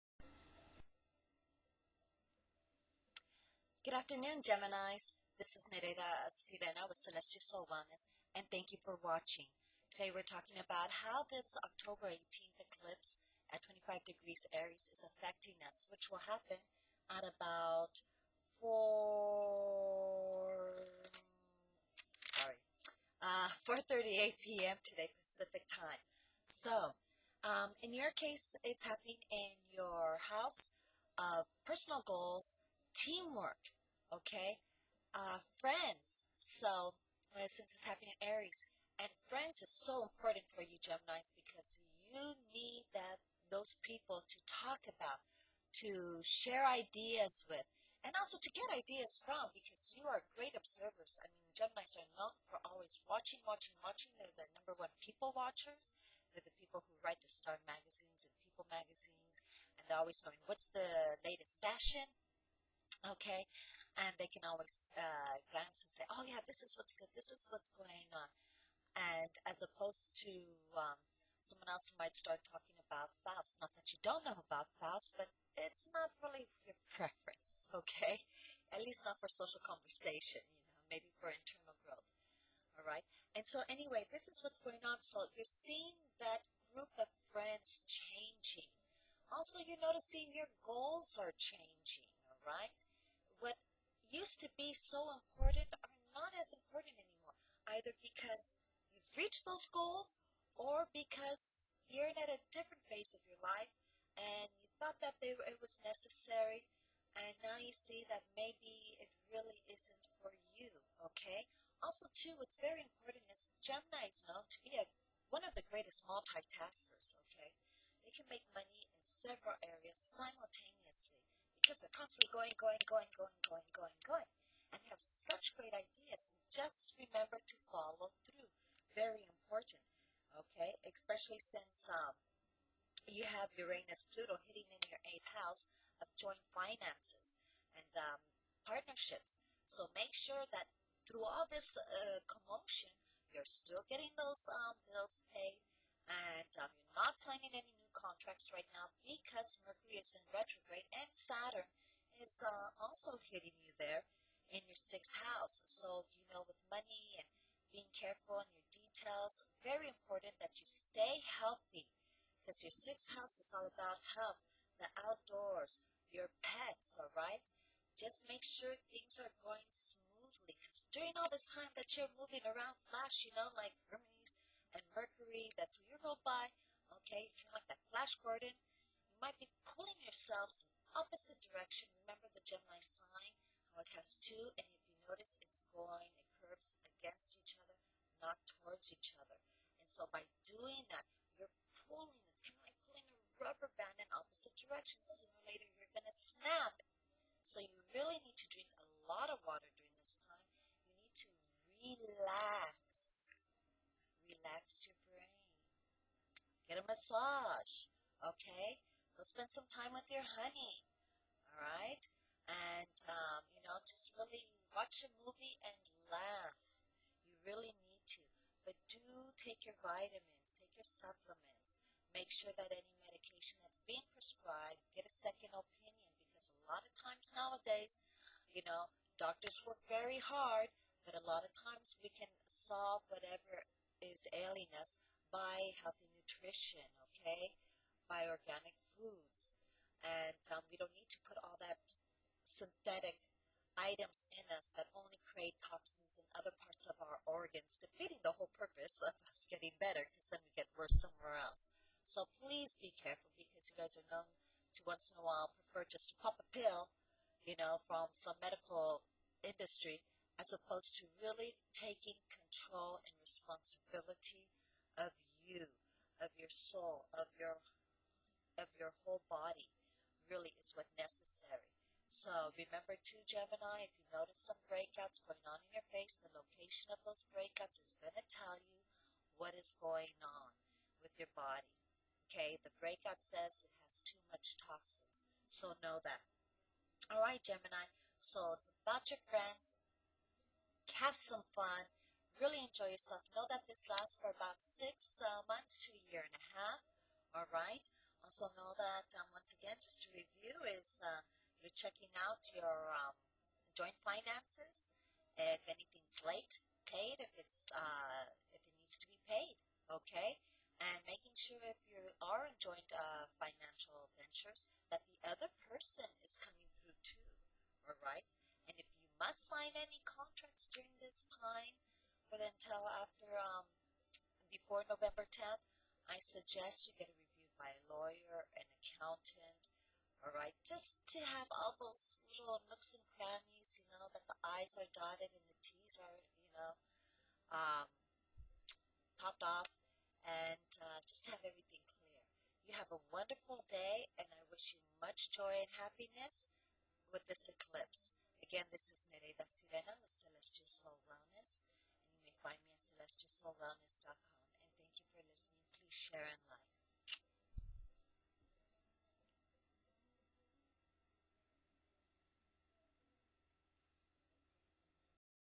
Gemini, October 18th, 2013 Lunar Eclipse
How it affects the Geminis in their 11th house, with the help of push and pull of Uranus and Pluto in the 8th house.  Reminder of Saturn in their 6th House, reinforced by Mercury.